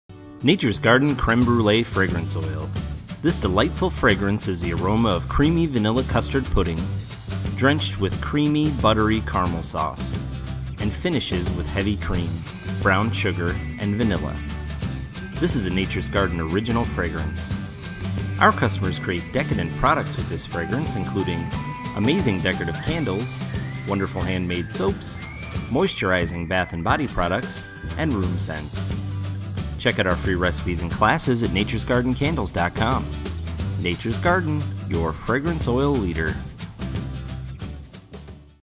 Creme Brulee Fragrance Oil- Natures Garden
Creme Brulee Fragrance Oil

This delightful fragrance oil by Natures Garden is the aroma of creamy vanilla custard pudding drenched with creamy buttery caramel sauce! Highly requested by customers! Fantastic! A Best Seller!

Nature’s Garden
42109 State route 18